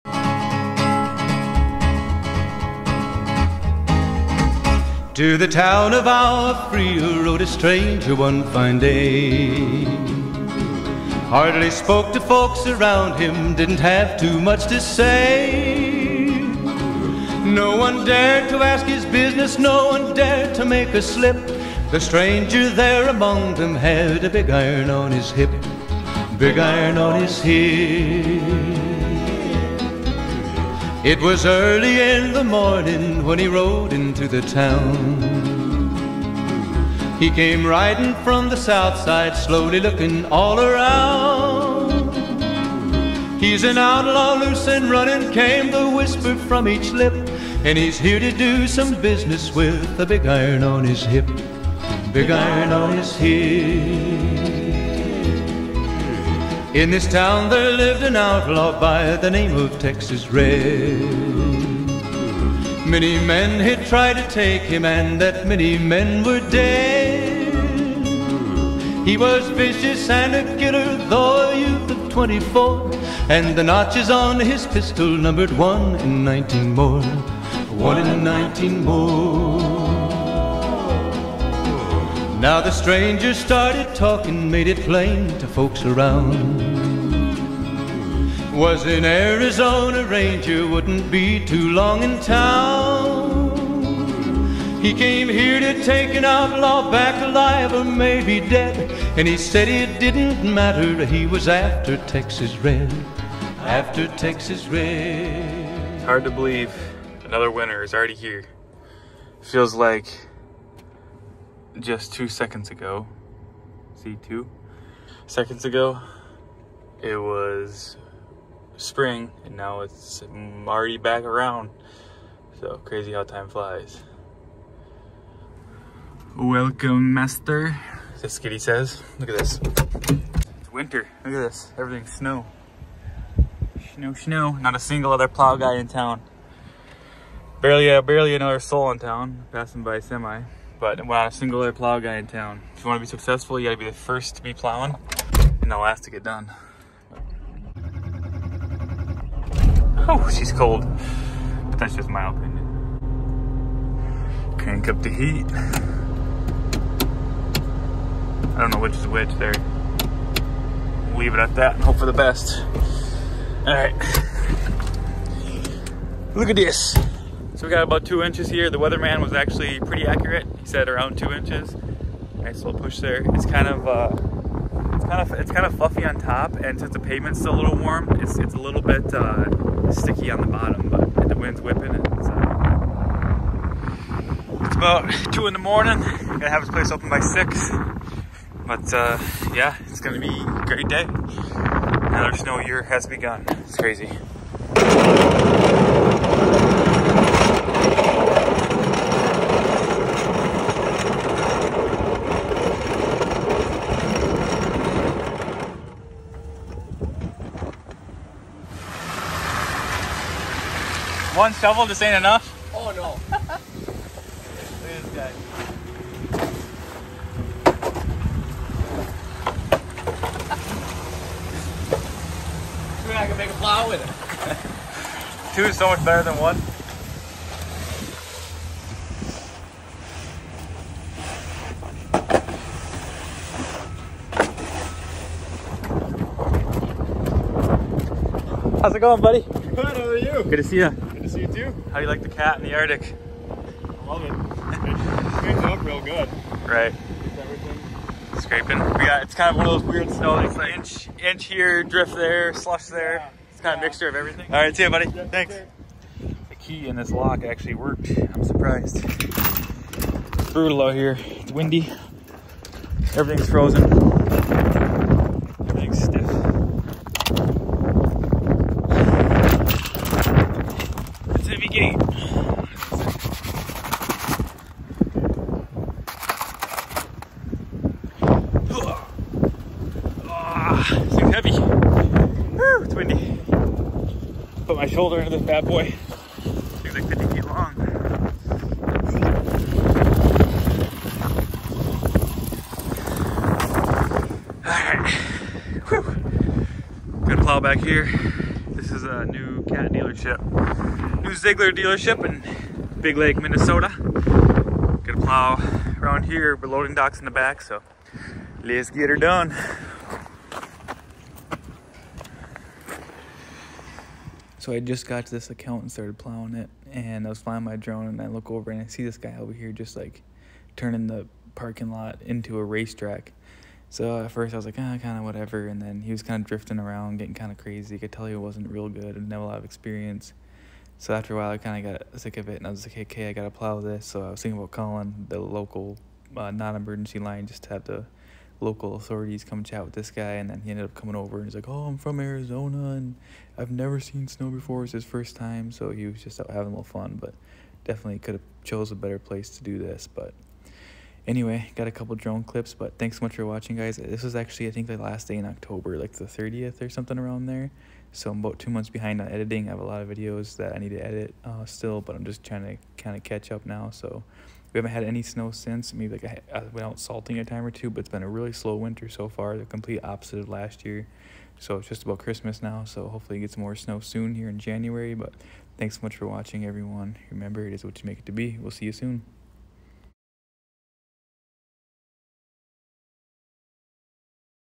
FIRST PLOWABLE SNOW EVENT OF THE 2023/2024 SEASON!!
In today’s video, we tackle the first snow event of the 2023/2024 season!